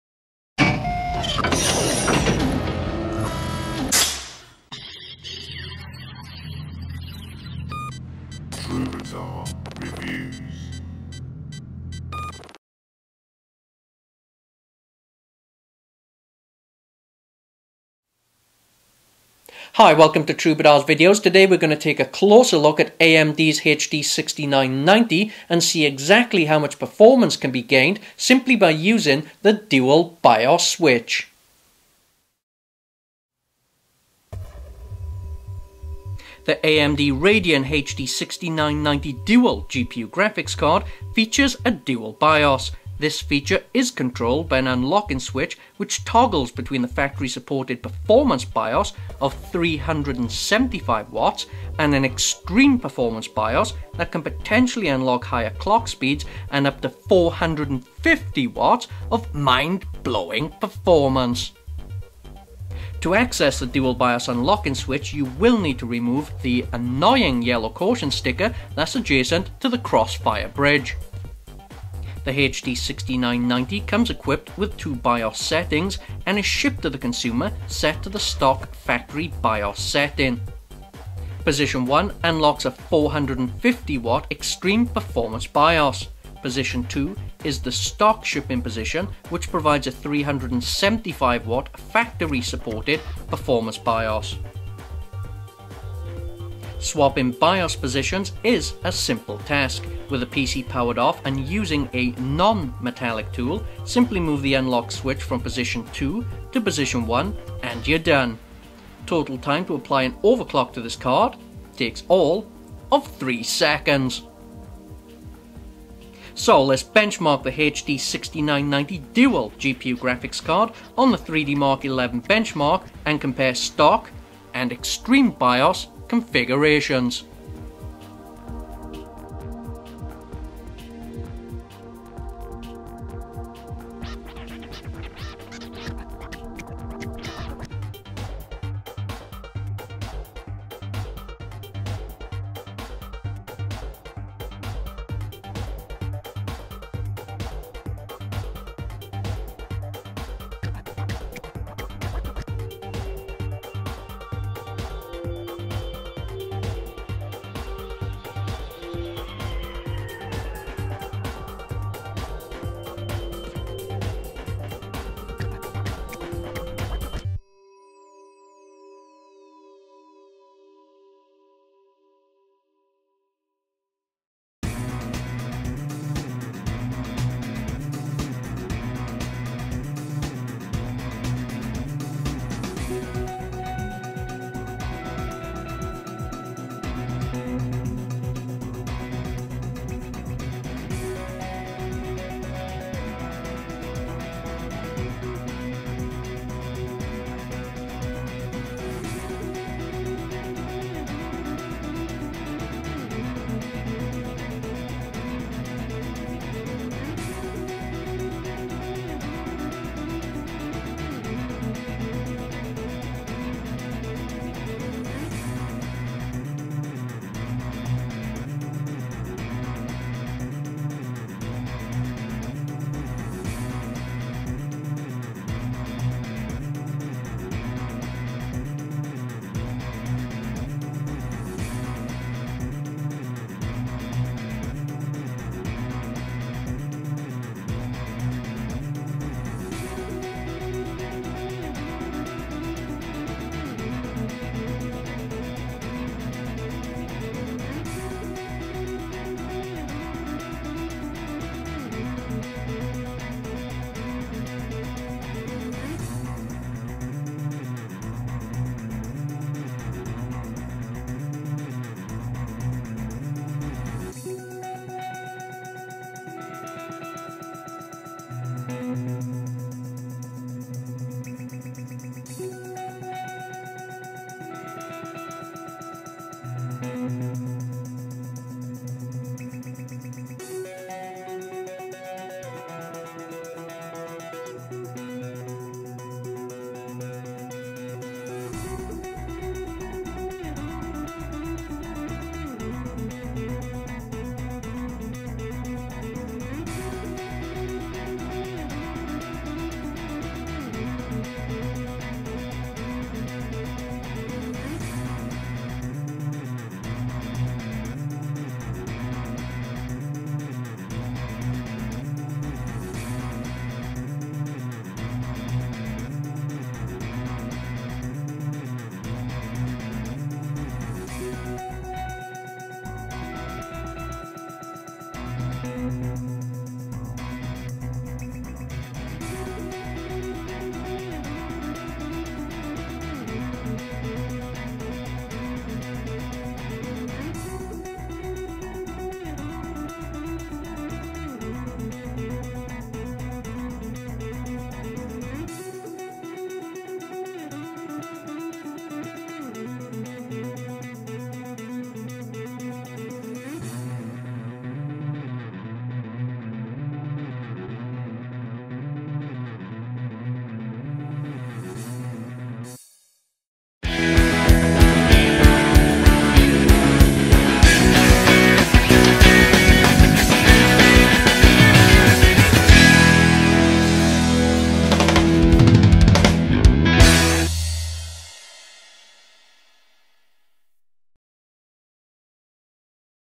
AMD HD6990, Dual Bios Performance comparison
Comparing the performance difference between the Stock Bios and Extreme performance Bios settiing on the AMD HD 6990.